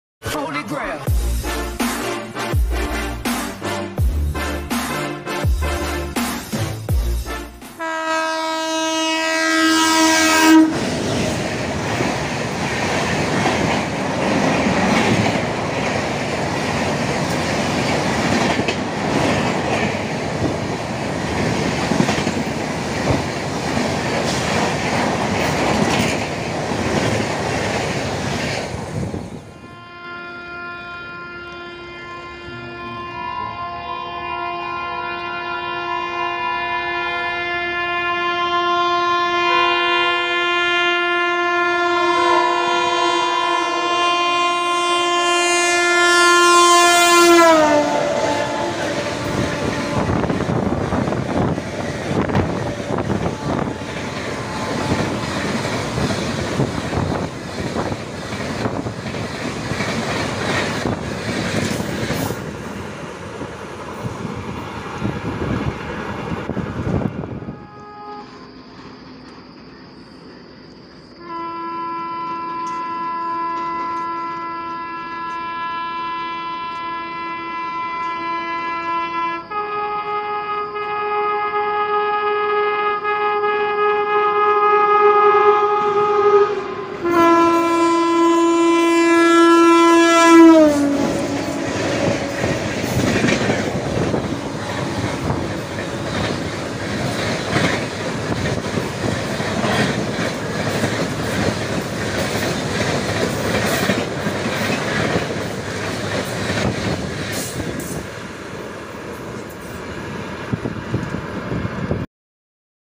Superfast LHB Trains with WAP 7 Engine! 3 सुपरफास्ट रेलगाड़ी रफ्तार में निकली
3 Superfast Trains in 1 video
1) 02724 Telangana Special hauled by LGD WAP 39156
2) 02752 Jammu to Nanded Humsafar Express Special led by Ajni WAP 7 37021
3) 02884 Chhattisgarh Sampark Kranti Festival Special hauled by Bhilai WAP 7
All hauled by WAP 7 Locomotive.

3 सुपरफास्ट रेलगाड़ी एक के बाद तेजी से WAP 7 इंजन के साथ निकलती हुई।

Indian Railways
Irfca
Indian trains
रेलगाड़ी
भारतीय रेल्वे
Ballabhgarh railway station